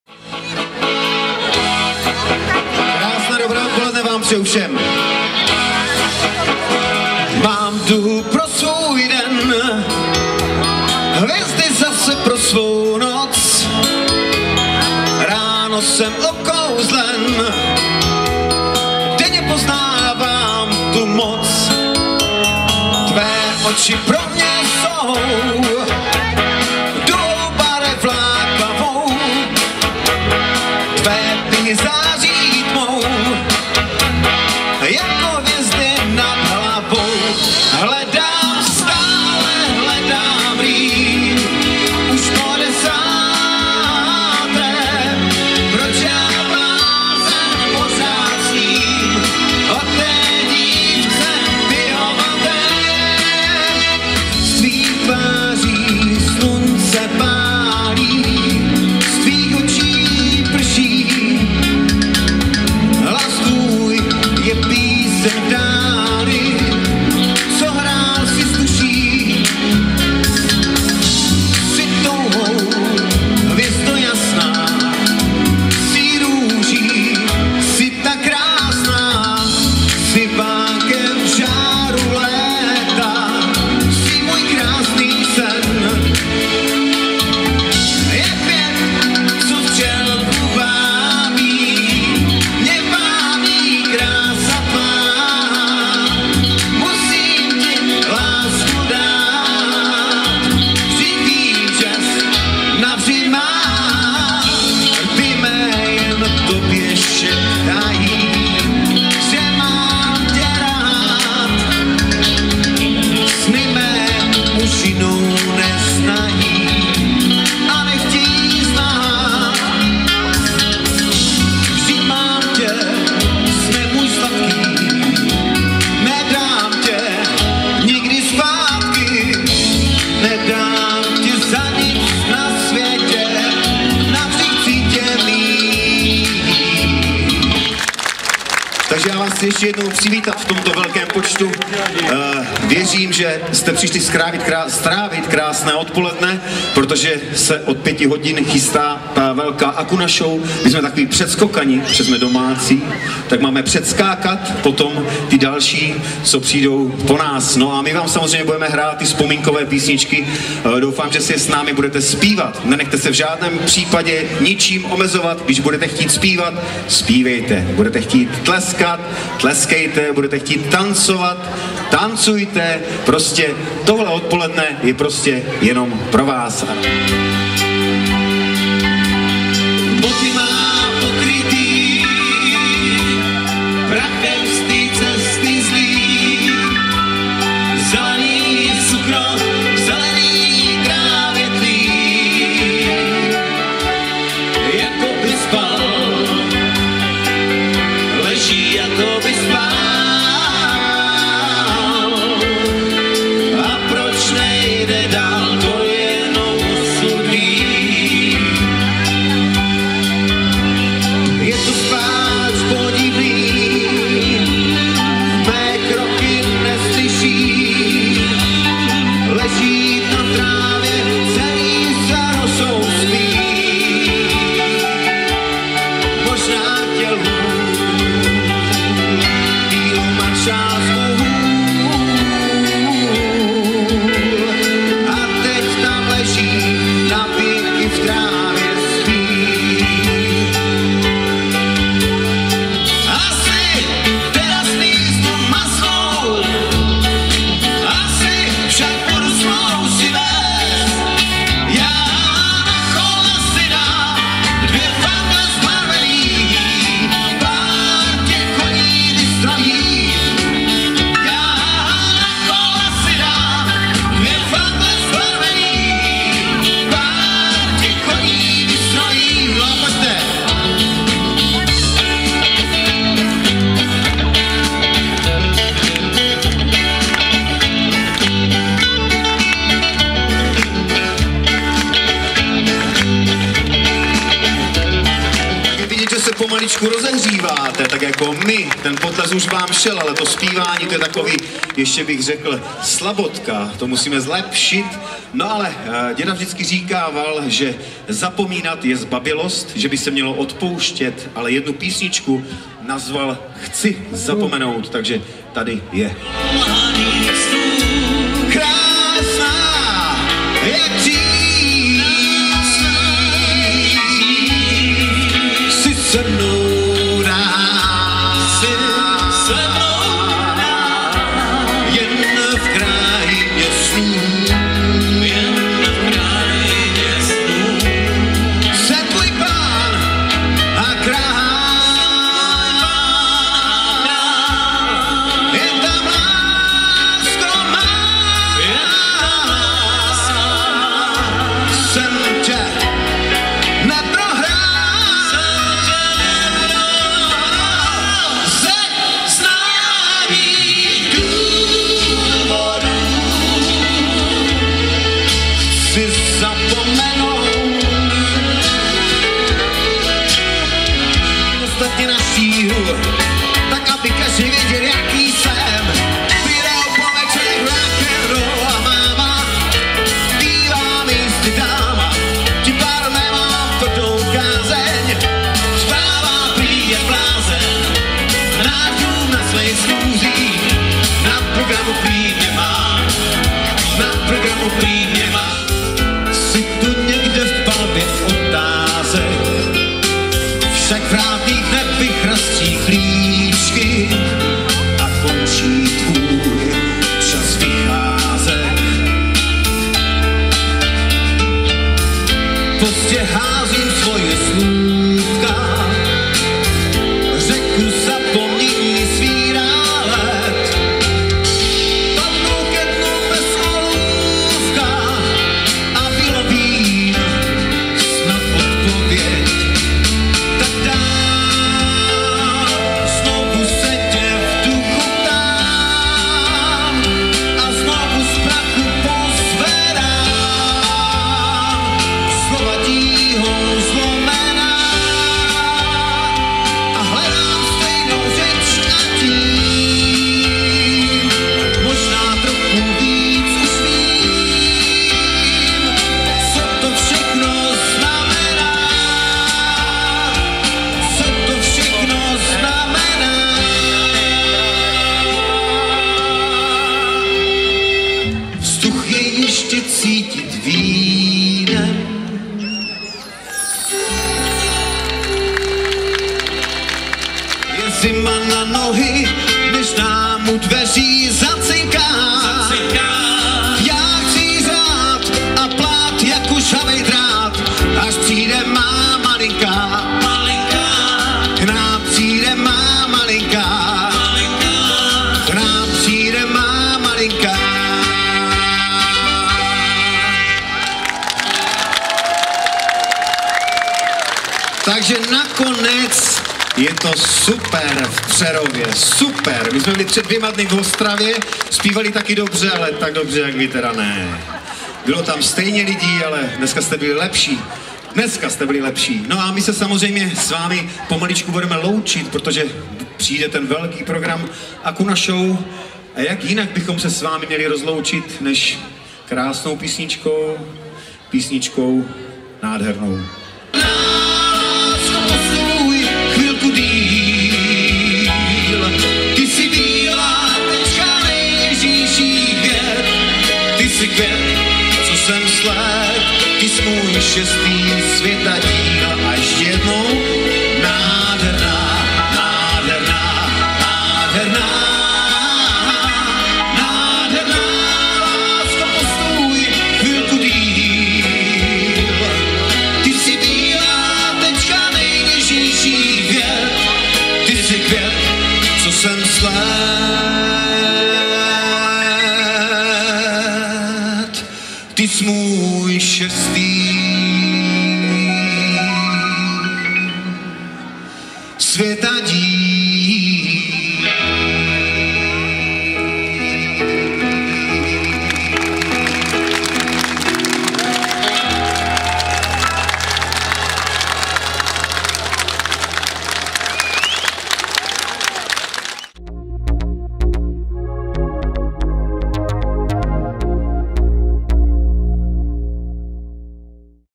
Akuna show 2010 - 1.část
Přinášíme vám sestřih videozáznamu z akce Akuna show 2010, která se konala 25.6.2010 v Přerově. Vzhledem k délce programu je video rozděleno do několika částí.

V této 1.části videozáznamu můžete zhlédnout zahajovací koncert odpoledního programu, ve kterém vystoupil Pavel Novák a skupina Family.